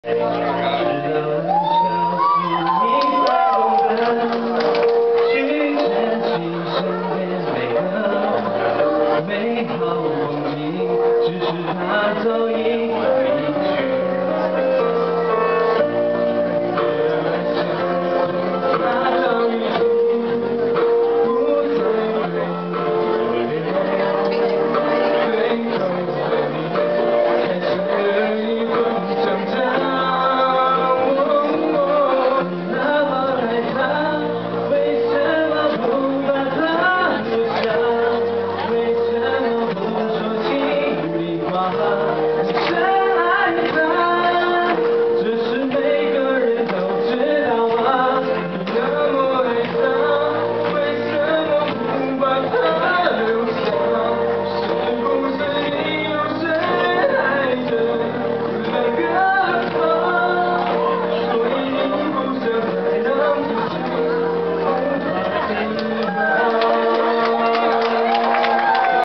CSSA CNY 12
2010 Chinese New Year Banquet & Show
Presented by CSSA, Missouri State University's Chinese Students and Scholars Association